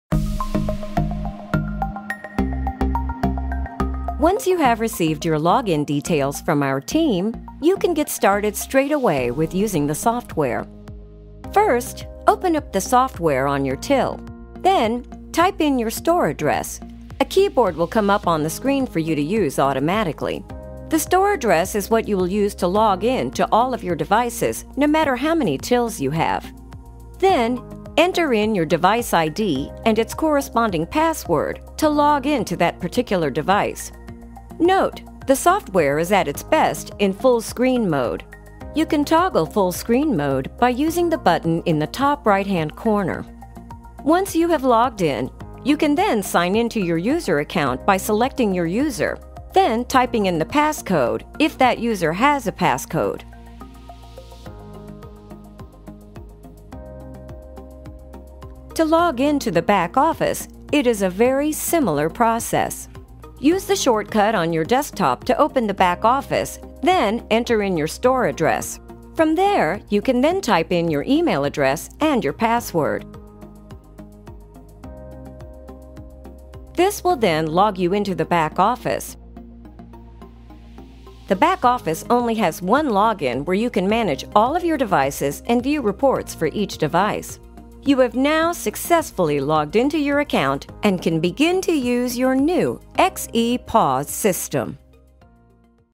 XEPOS Software Logging in
In this video we will walk you through the XEPOS Software Logging in.

One of the first steps to use an Epos System is to enter your information on the login page. XEPOS Software has two separated login page for the Front-office and the Back-office.

Topics Covered:
- Front-office Software Login
- Back-office Software Login